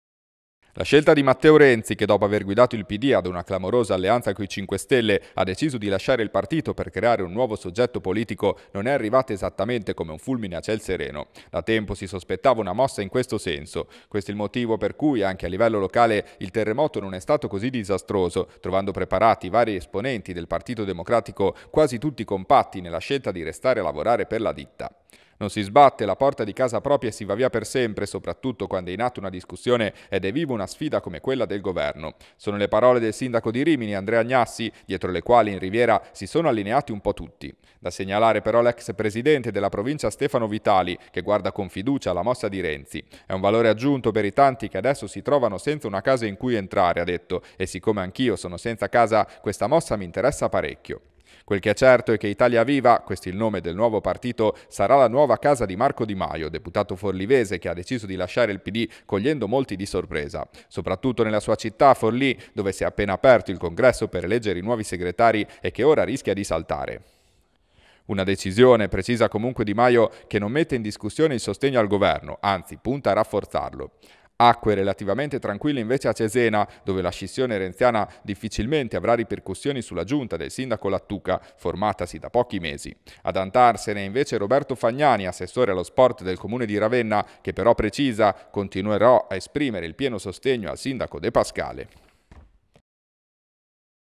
ROMAGNA: La scissione di Renzi agita le acque nel Pd, Di Maio lascia il partito | VIDEO
L’uscita di Matteo Renzi dal Pd agita le acque del governo e della politica nazionale. Intanto anche la livello regionale si compongono i nuovi schieramenti. Sentiamo chi va e chi resta.La mossa di Matteo Renzi, che dopo aver guidato il PD ad una clamorosa alleanza coi 5 Stelle ha deciso di lasciare il partito per creare un nuovo soggetto politico non è arrivata esattamente come un fulmine a ciel sereno. Da tempo si sospettava una mossa in questo senso. Questo il motivo per cui, anche a livello locale, il terremoto non è stato così disastroso, trovando preparati i vari esponenti del partito democratico, quasi tutti compatti nella scelta di restare a lavorare per la ditta. “Non si sbatte la porta di casa propria e si va via per sempre, soprattutto quando è in atto un discussione ed è viva una sfida come quella del governo” Sono le parole del sindaco di Rimini Andrea Gnassi, dietro alle quali si sono allineati un po’ tutti. Da segnalare, però, l’ex presidente della provincia Stefano Vitali che guarda con fiducia alla mossa di Renzi “E’ un valore aggiunto per i tanti che adesso si trovano senza una casa in cui entrare – ha detto - e siccome anch’io sono un senza casa questa mossa mi interessa parecchio”. Quel che è certo è che Italia Viva, questo il nome del nuovo partito, sarà la nuova casa di Marco Di Maio, deputato forlivese che a deciso di lasciare il Pd cogliendo molti di sorpresa. Soprattutto nella sua città, Forlì, dove si è appena aperto il congresso per eleggere i nuovi segretari e che rischia ora di saltare “Una decisione- precisa comunque Di Maio – che non mette in discussione il sostegno al governo, anzi punta a rafforzarlo”. Acque relativamente tranquille invece a Cesena, dove la scissione renziana difficilmente avrà ripercussioni sulla giunta del sindaco Lattuca, formatasi da pochi mesi. Ad andarsene è invece Roberto Fagnani, assessore allo sport del Comune di Ravenna che però precisa “Continuerò a esprimere il pieno sostegno al sindaco De Pascale”